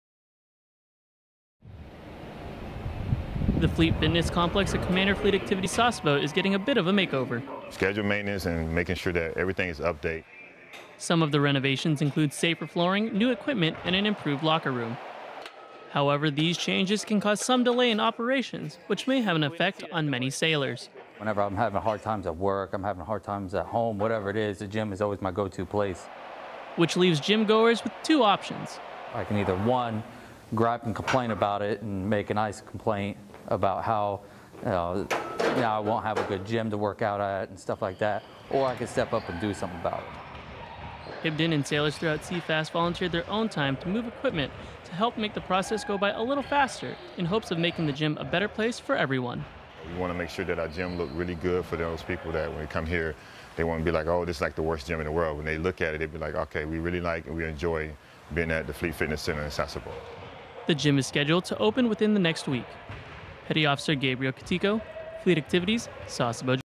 Commander Fleet Activities Sasebo Gym Renovations
Sailors at Commander, Fleet Activities Sasebo help personnel at the Fleet Fitness Complex to speed up building renovations.
(Video by Petty Officer 3rd Class Gabriel Kotico)